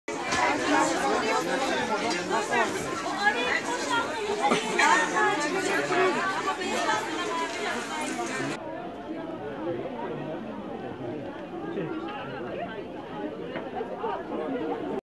Sakarya Çarşamba Pazarı'nda bir kadın, kocası tarafından herkesin gözü içinde dövüldü
Edinilen bilgiye göre, Erenler İlçesi'nde kurulan Çarşamba Pazarı'nda bugün öğle saatlerinde karı-koca arasında boşanma olayı nedeniyle tartışma çıktı. Tartışmanın büyümesi üzerine ismi öğrenilemeyen kişi, herkesin gözü önünde eşine dayak attı. Aldığı darbelerle yere yığılan kadın yaralanırken, öfkeli koca olay yerinden hızla uzaklaştı.

Sakarya Rehberim resmi youtube hesabıdır. Kanalımızda güncel ve gündem olan son dakika haberlerini paylaşmaktayız.